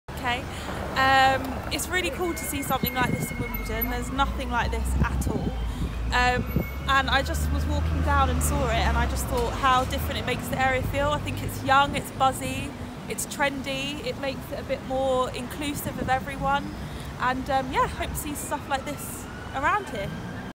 "It's Oh So Quiet"
Public Reaction to the art installation "It's Oh So Quiet" in Wimbledon Town Centre, created by Love Wimbledon, in collaboration with artist Louis Masai and local photographer and culture campaigner Cindy Sasha.